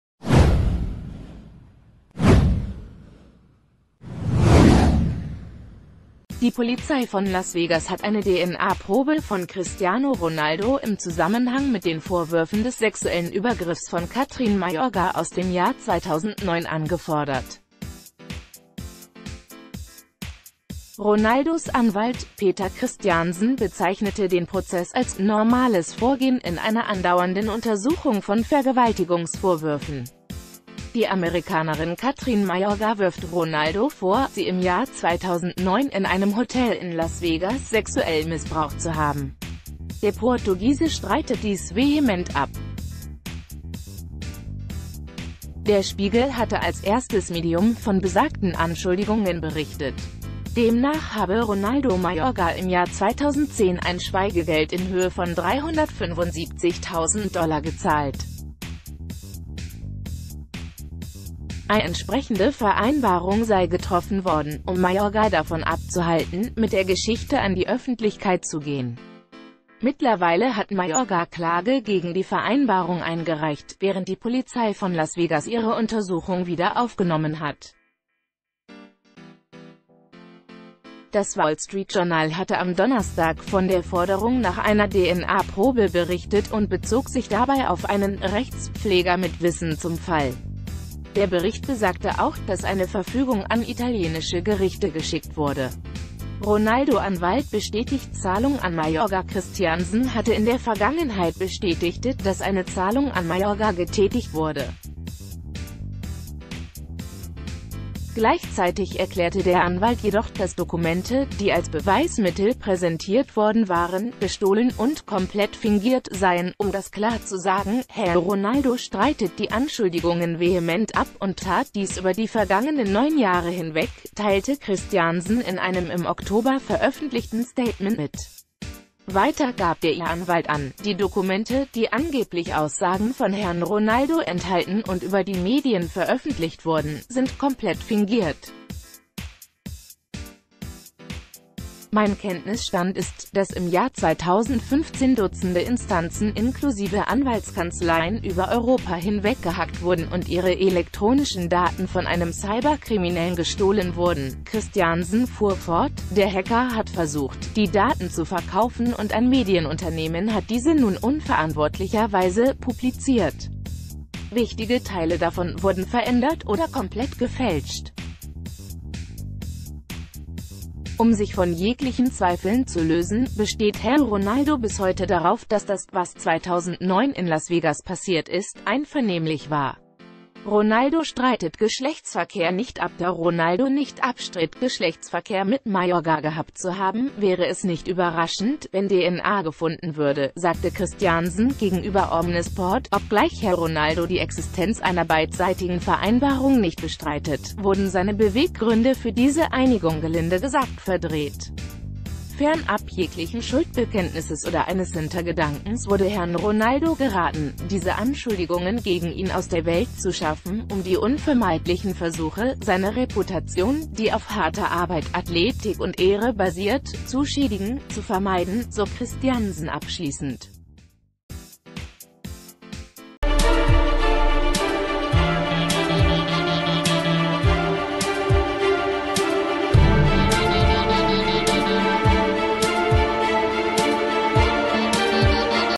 Vergewaltigungsvorwürfe gegen Ronaldo: Polizei fordert DNA-Probe an
Die Polizei von Las Vegas hat eine DNA-Probe von Cristiano Ronaldo im Zusammenhang mit den Vorwürfen des sexuellen Übergriffs von Kathryn Mayorga aus dem Jahr 2009 angefordert. Ronaldos Anwalt Peter Christiansen bezeichnete den Prozess als "normales Vorgehen" in einer andauernden Untersuchung von Vergewaltigungsvorwürfen. Die Amerikanerin Kathryn Mayorga wirft Ronaldo vor, sie im Jahr 2009 in einem Hotel in Las Vegas sexuell missbraucht zu haben. Der Portugiese streitet dies vehement ab. Der Spi...